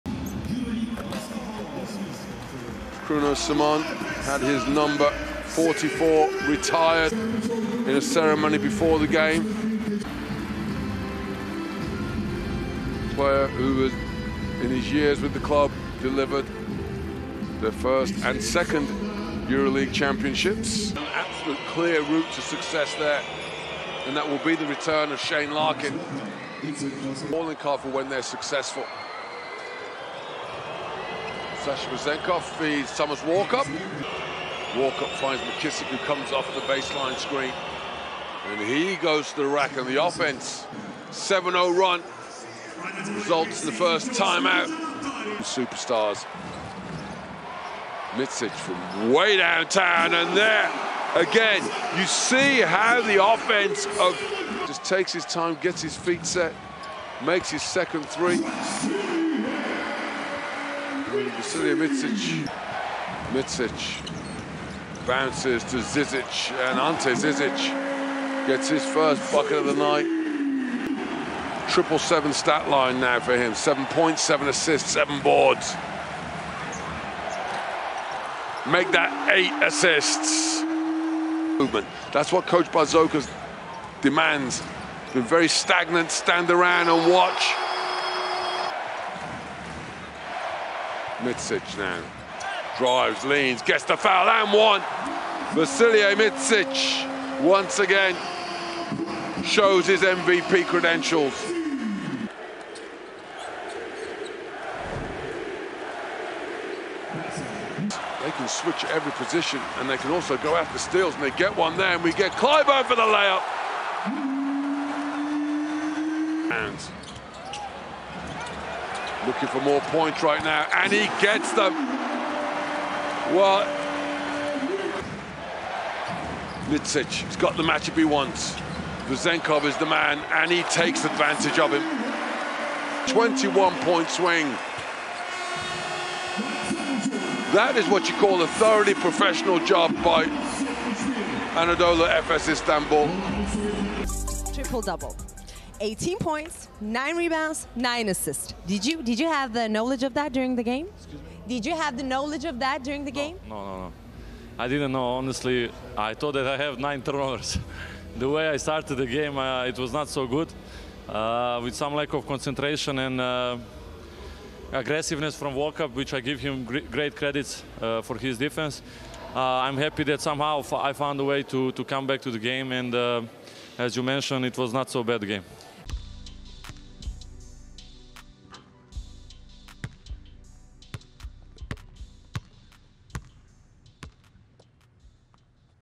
MVP Gets Benched In 2 Mins... Then Drops Near Triple Double
Despite a tough start to the game, Vasilije Micic was very near a rare achievement in the EuroLeague as only 1 rebound and 1 assist separated the guard from a triple-double (18 PTS, 9 REB, 9 AST). Meanwhile, Anadolu Efes Istanbul overcame Olympiacos Piraeus in EuroLeague's Round 11 after a solid run in the fourth quarter.